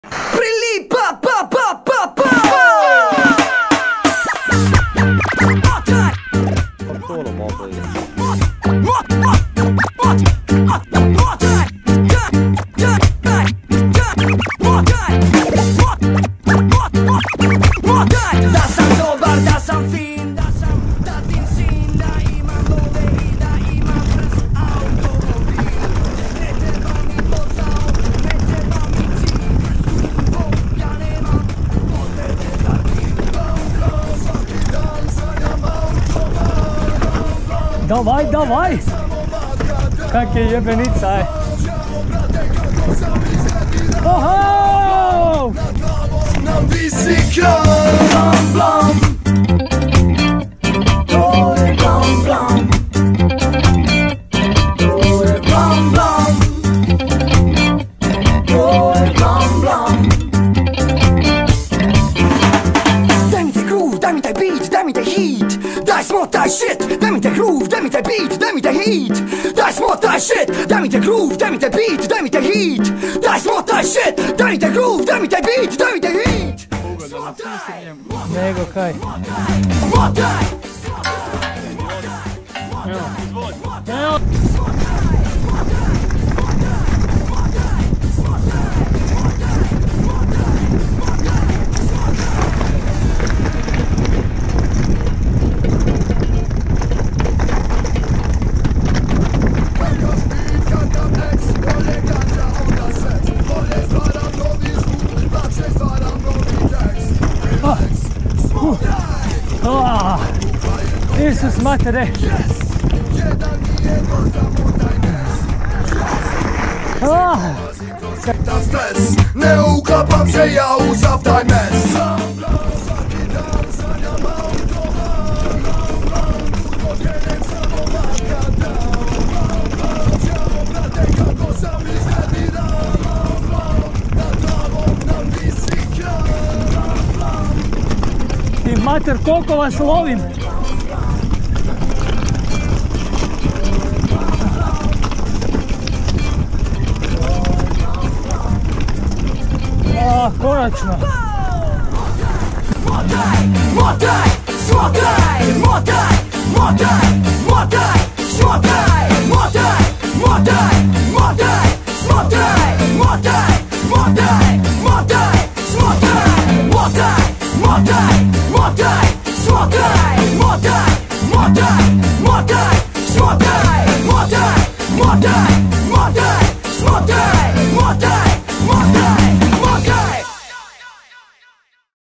Baška mtb
Neznam po koji put smo već bili na Krku i prašinarili po Baškoj ali svaki put je dobro i pemo opet jer fulali smo povrat i nismo imali vremena sve trailove odvoziti!